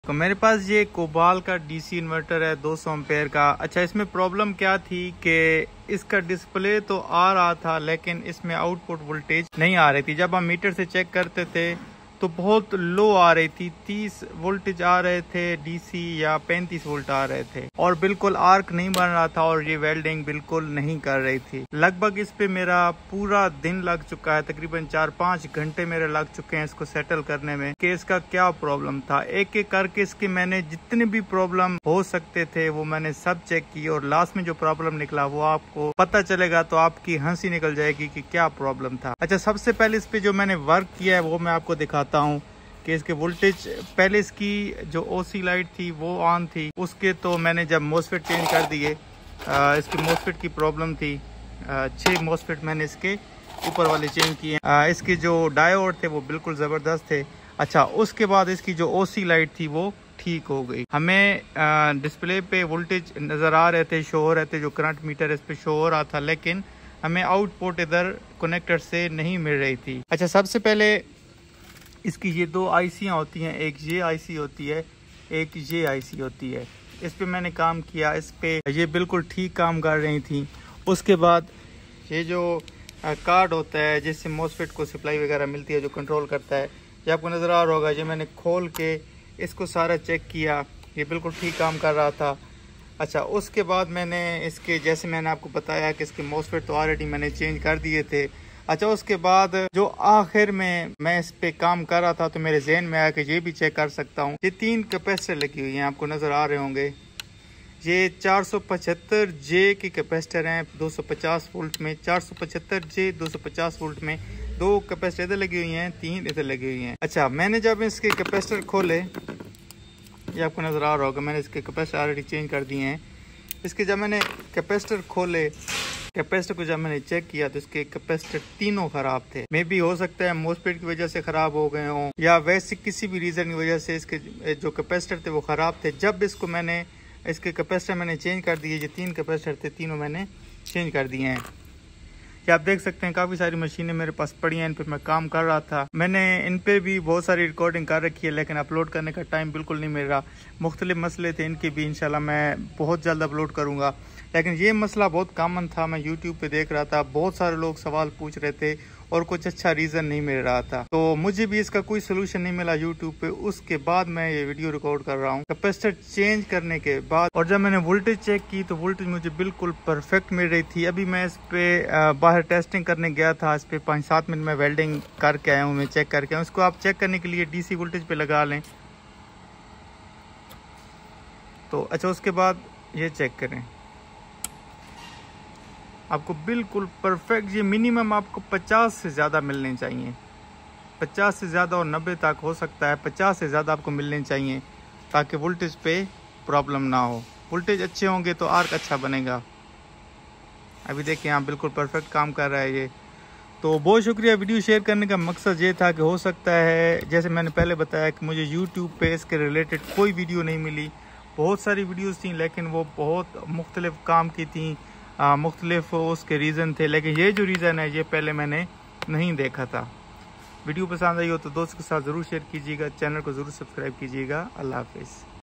How to Repair a Welding machine with low voltage output || display working || mosfet ok
Title: Welding Machine Repair | Display OK but Low Voltage Output

Description:
In this video, we’ll walk you through troubleshooting and fixing a common issue with welding machines: when the display is working fine, but the output voltage is too low. We’ll cover possible causes such as faulty components, loose connections, or issues with the rectifier and transformer. Whether you’re an electrician or a DIY enthusiast, this step-by-step guide will help you diagnose the problem and get your welding machine running at full power again.